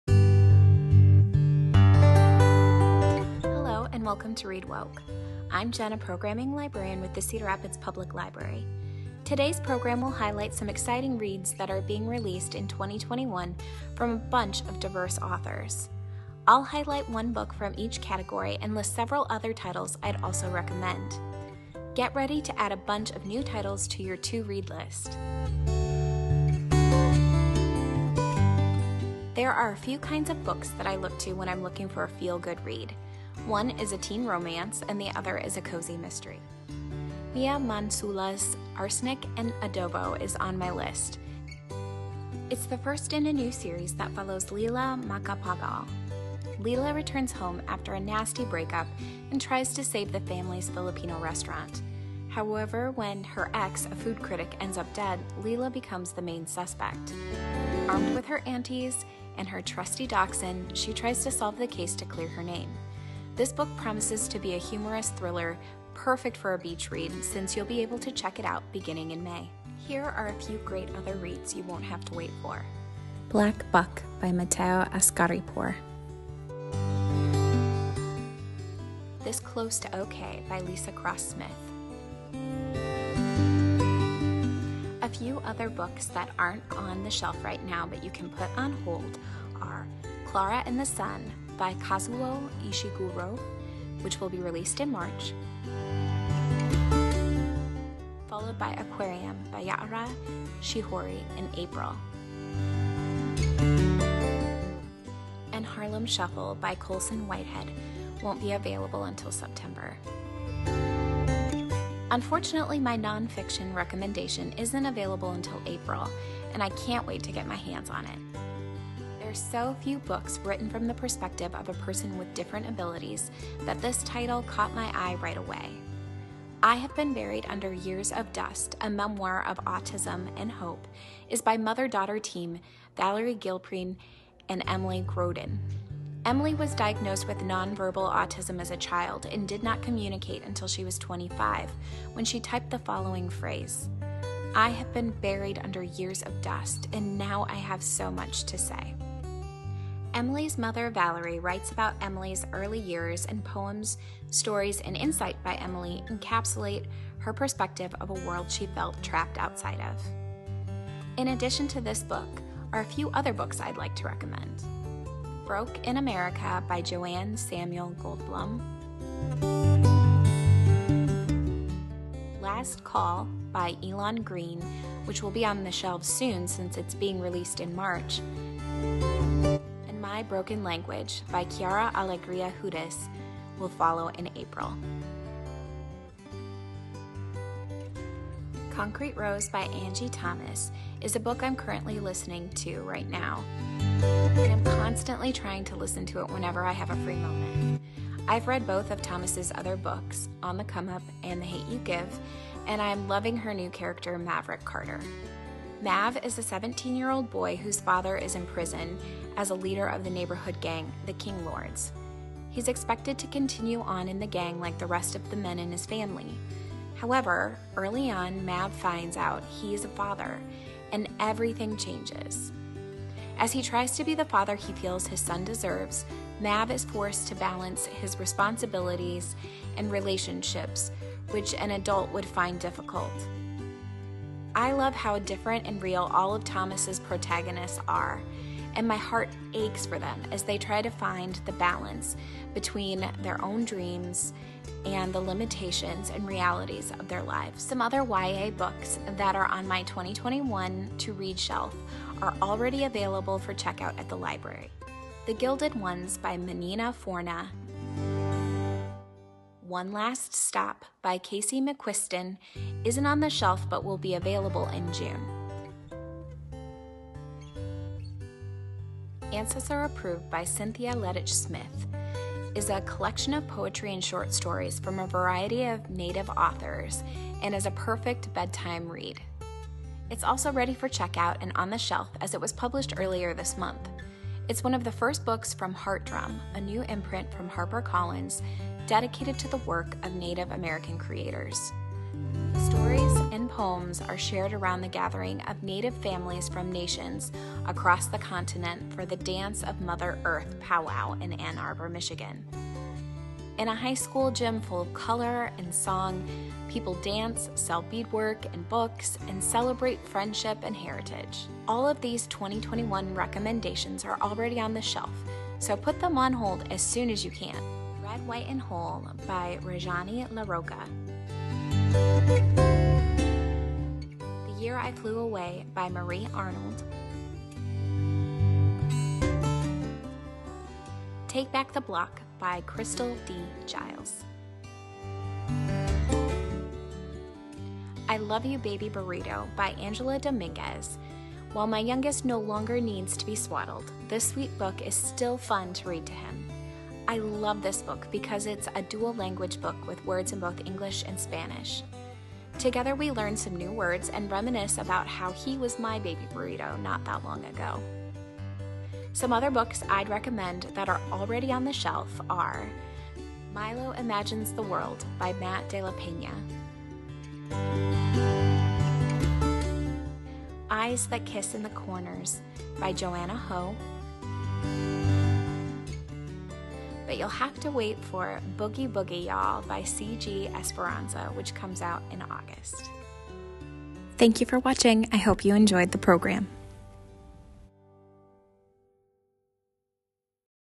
Read Woke: Buzzworthy New Woke Releases of 2021 for all Ages
Let us help you fill out your 2021 reading list with our recommendations for all readers.